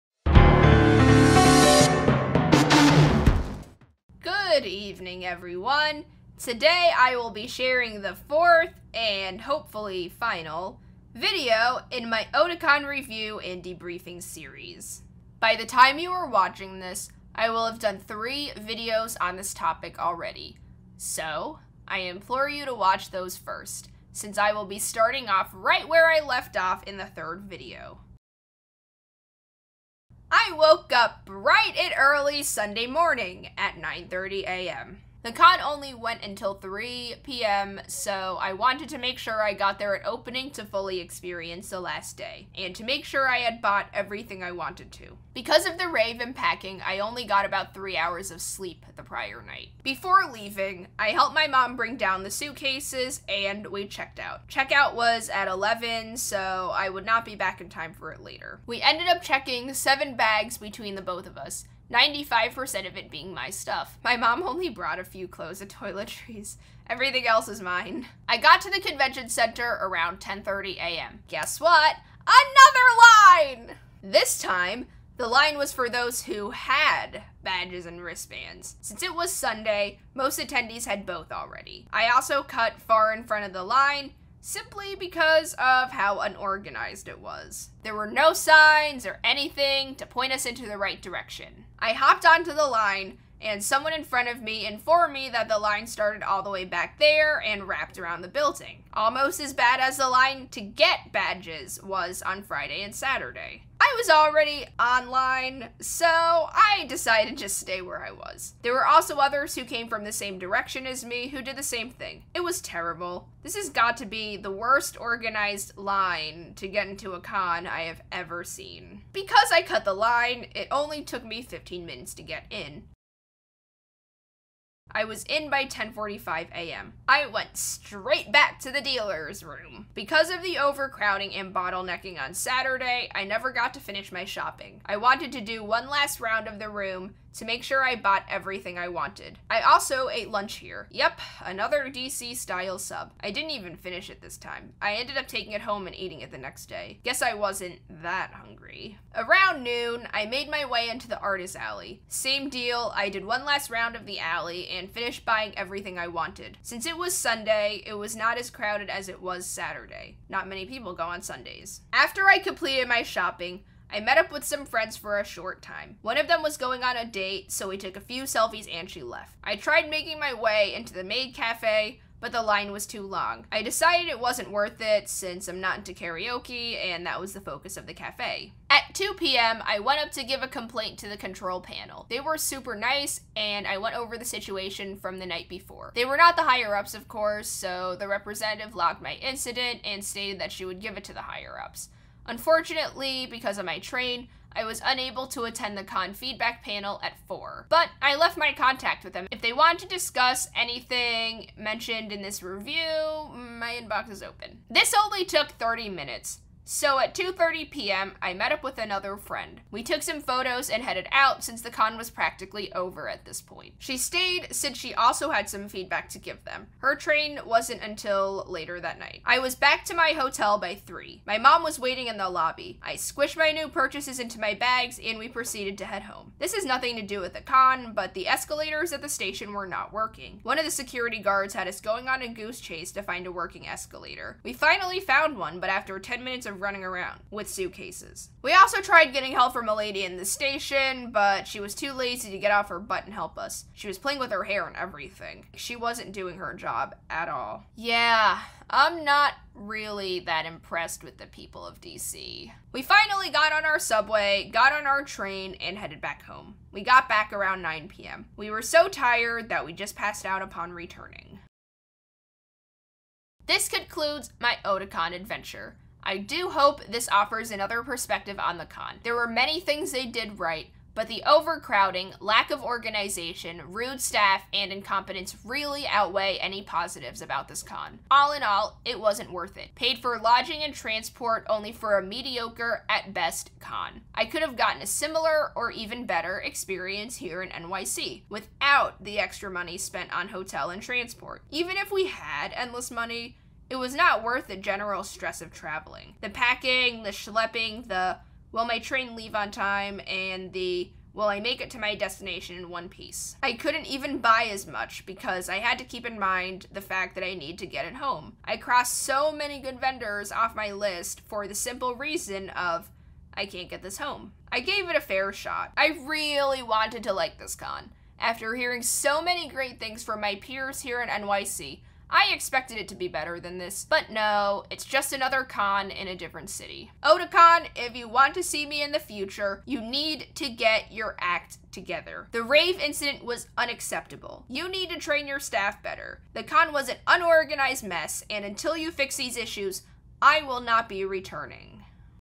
Otakon Review Part IV FINALE - The Aftermath/Conclusion
Hello everyone! Welcome back to my first ever con review video series. This is a big one. This is part four of a four (maybe five) part series recapping my experience at Otakon, which took place at the Walter E. Washington Convention Center in Washington DC. In this video, I will be going over the final day of Otakon. Including Linecon and the aftermath of the previous night. I also revisit my gripes with this con, and why I won't be returning. This video is short since it's only meant to tie up any loose ends I may have missed in the previous three videos. As far as a follow up? Maybe a "lessons learned" video, but I do hope to be done with this ordeal after this video.

-Firestar